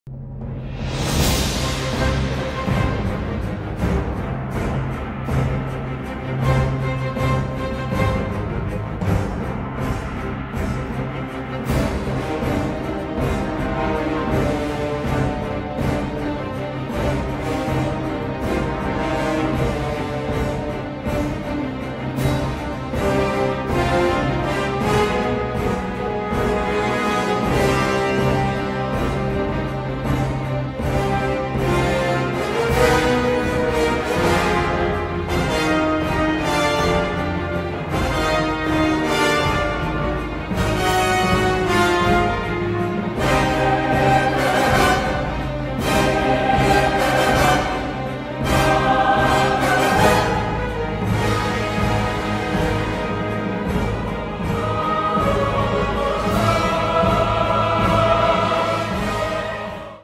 WoW Burning Crusade Classic Mural, Sydney
The talented team at Apparition Media painted this art from Burning Crusade Classic to celebrate the launch of WoW Classic's first expansion!